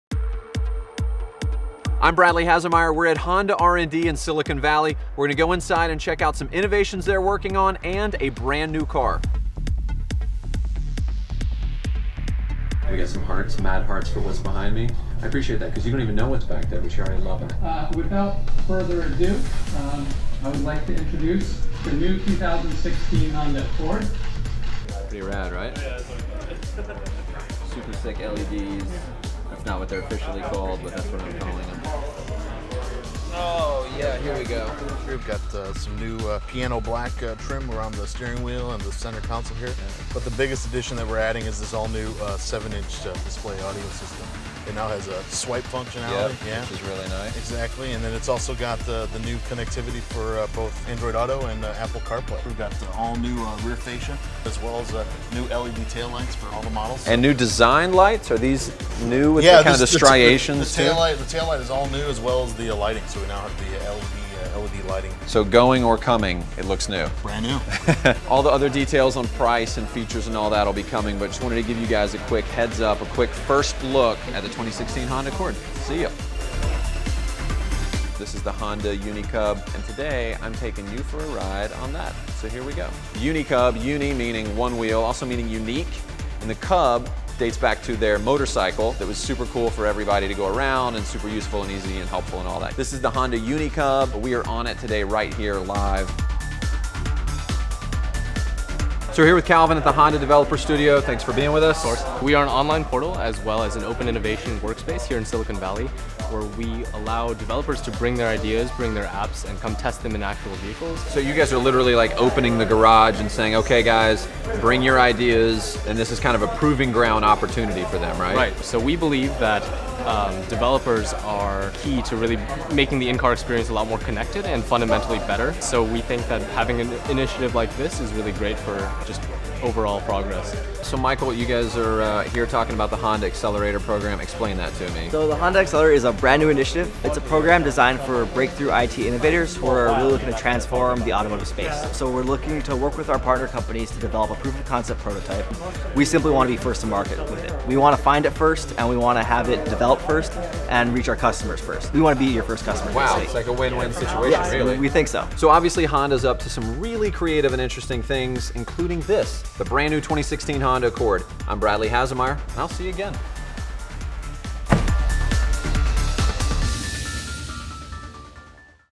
The Grand Opening of the Honda Silicon Valley R&D Facility.
Catch all the amazing highlights from the grand opening of the new Honda Silicon Valley R&D facility. There’s footage from our live stream of the 2016 Accord reveal on Periscope, a behind-the-scenes peek at the Honda Developer Studio, tech demos and more!